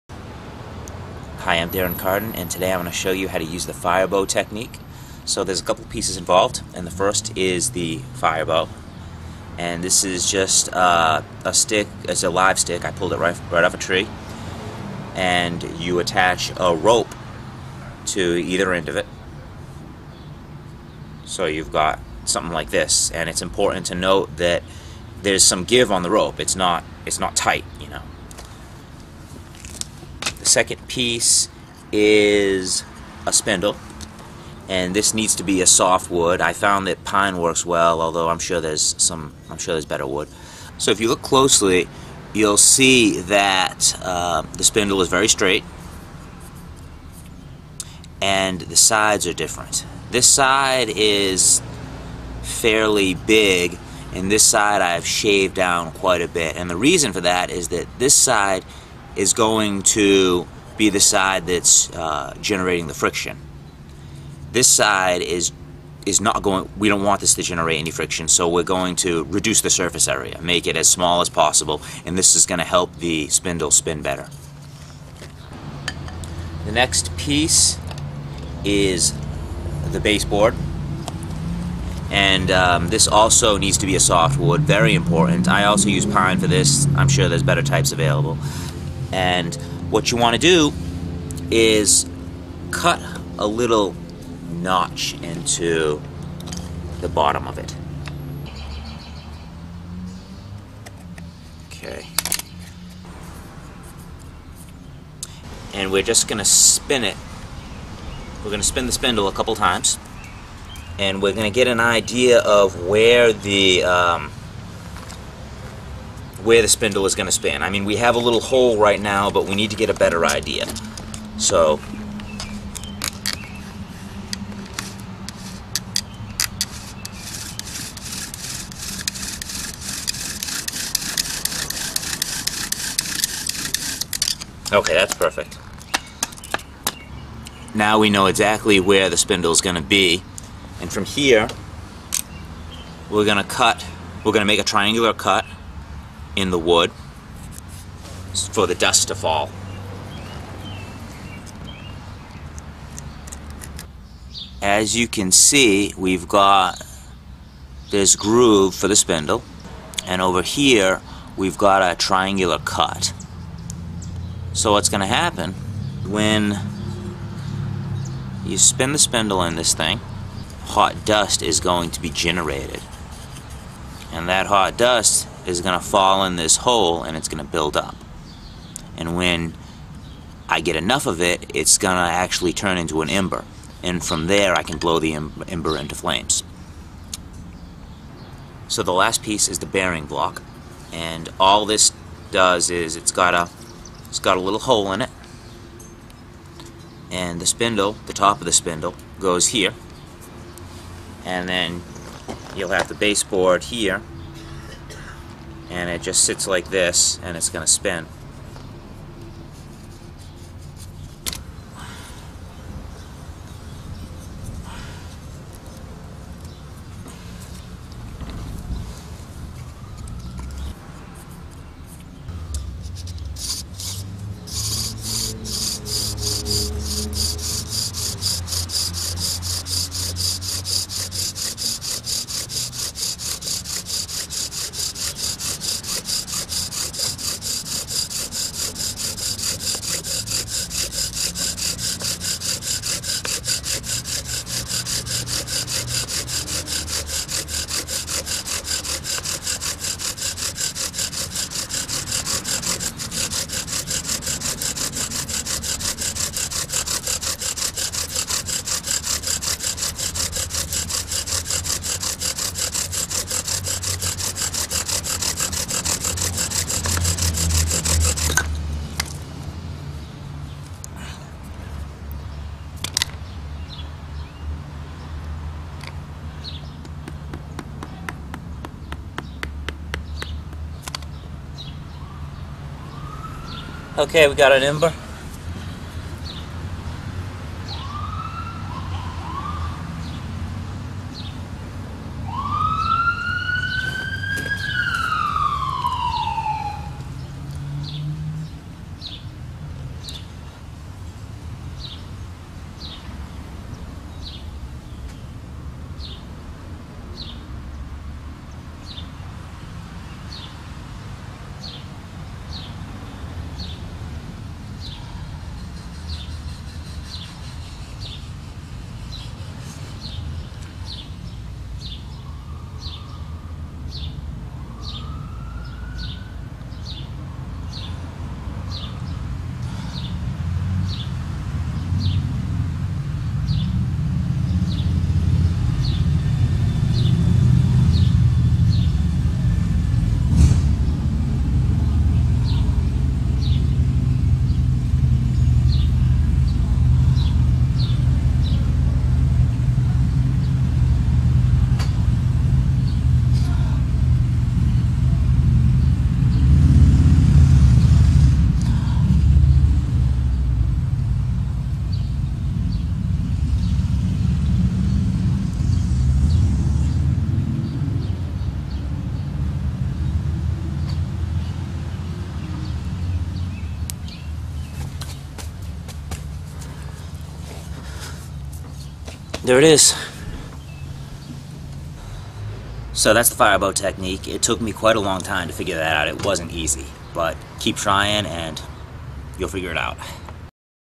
Firebow Technique
Demonstration on how to make a fire using the firebow technique.